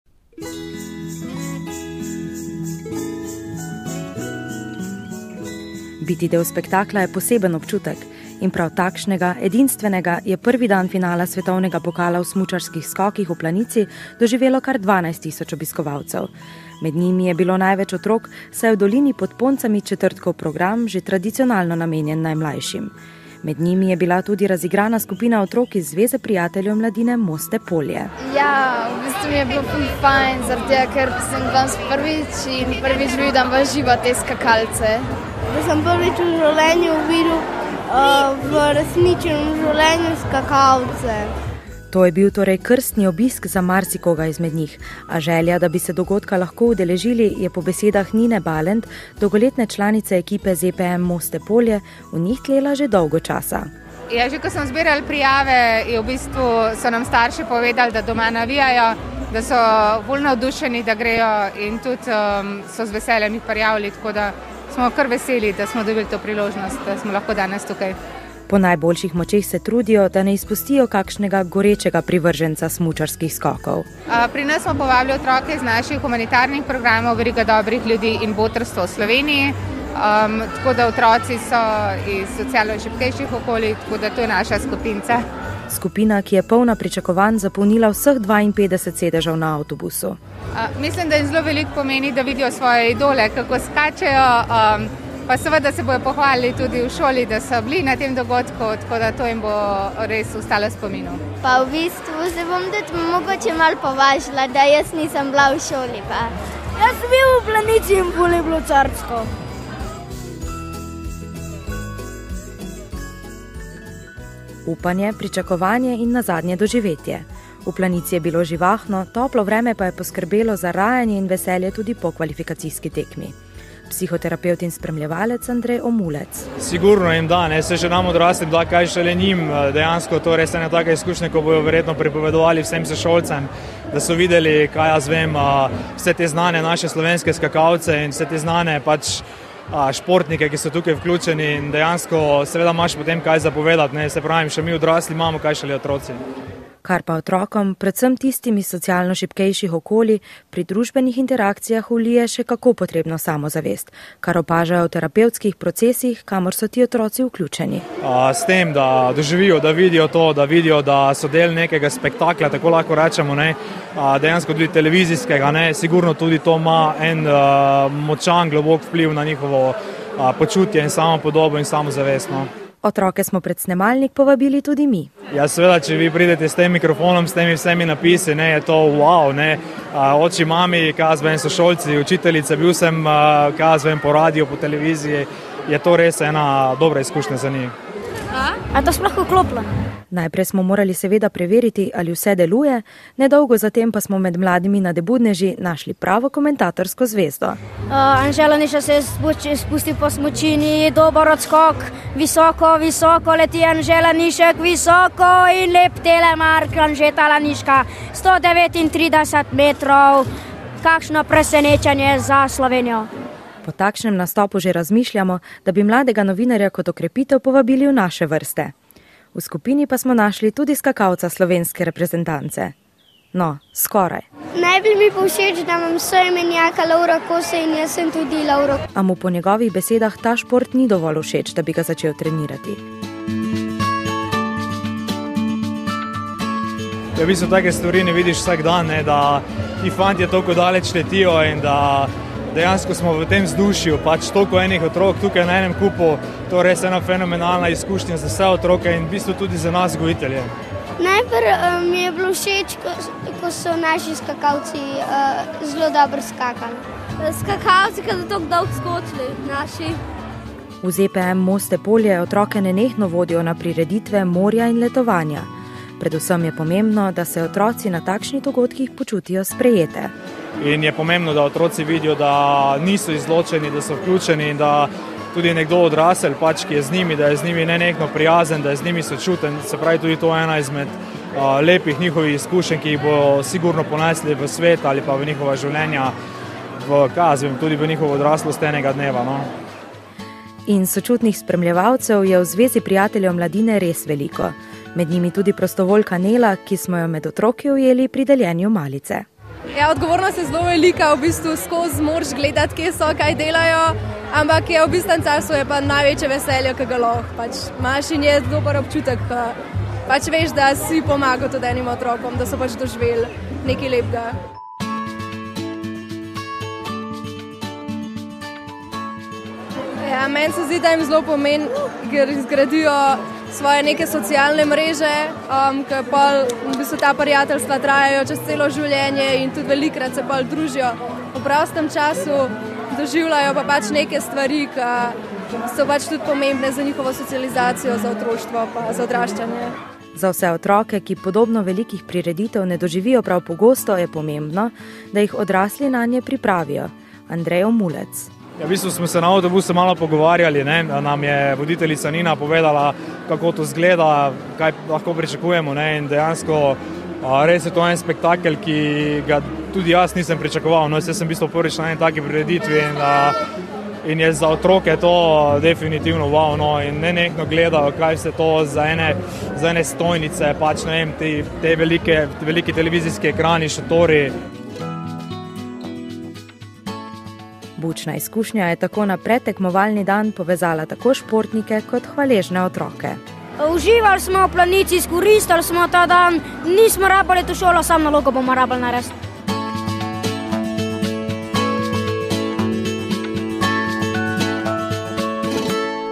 Biti del spektakla je poseben občutek
Planiški četrtek je tradicionalno namenjen otrokom. Tudi letos se ga je udeležilo več tisoč otrok z vseh koncev Slovenije in tudi iz zamejstva, mnogim so obisk omogočili različni donatorji. Med tistimi, ki so glasno spodbujali slovenske skakalce, so bili tudi otroci iz programa Botrstvo. Avtobusi so v dolino pod Poncami prispeli iz Maribora, Velenja, Novega mesta, Cerknega, Idrije, Sevnice, Ljubljane, Šentjurja, Ptuja, Ajdovščine in Metlike.

Kako so se imeli in kaj se jim je še posebej vtisnilo v spomin?